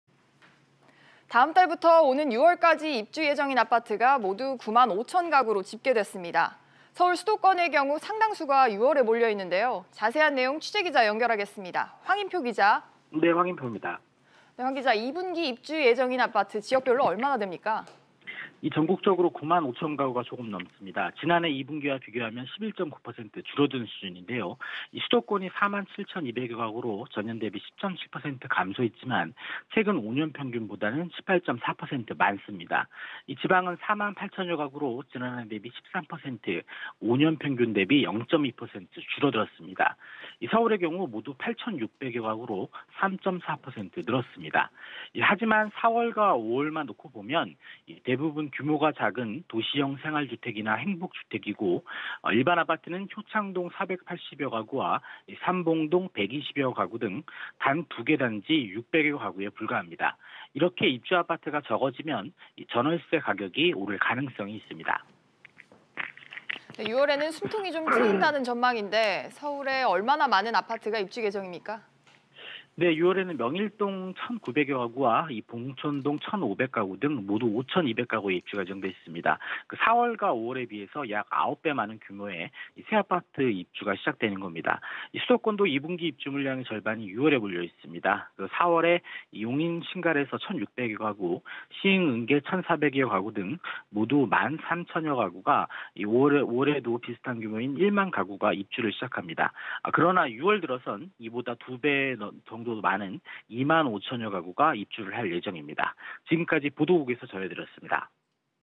봄 이사철 ‘가뭄’…올 2분기 입주예정 아파트 9.5만 가구
다음 달부터 오는 6월까지 입주 예정 아파트가 모두 9만5000가구로 집계됐습니다.

서울 수도권의 경우 상당수가 6월에 몰려있는데요.

◇백브리핑 시시각각 (월~ 금 오전 12시 ~12시 30분, 앵커: 안지혜)
◇출연: 황인표 기자